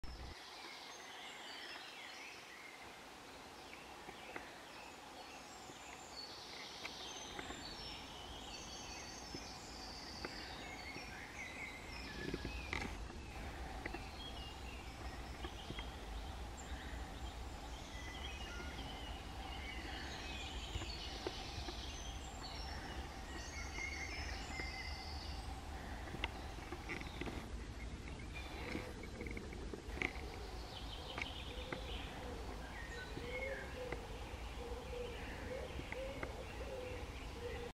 Great spotted woodpecker chick
This is one of two GSW nests we where able to find last June